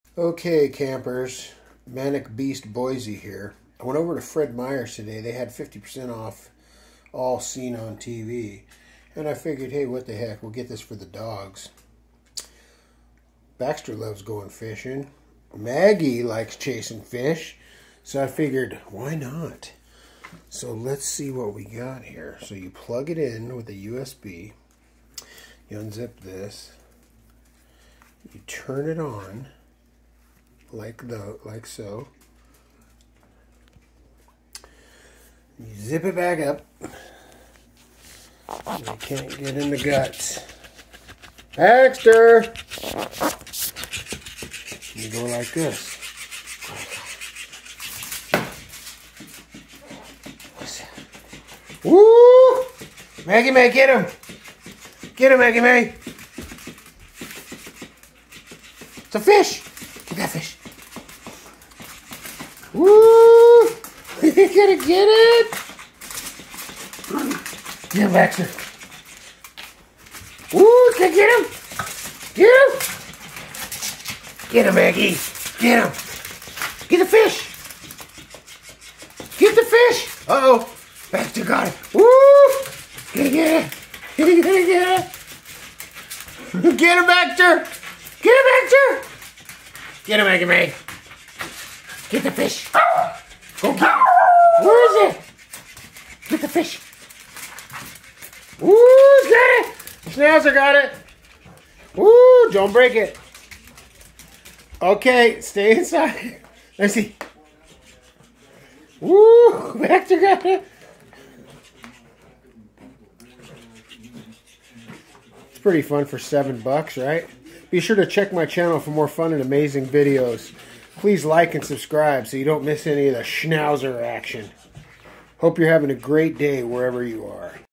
"Flippity Fish" cat toy review by Mini Schnauzer.. 4/2021~ ManicBeastBoise
Fred Meyer had 50% off as seen on TV items so I figured I'd get the flippity fish cat toys so my two mini schnauzers could play with it! please check my channel for more fun and amazing videos! Google search manic Beast Boise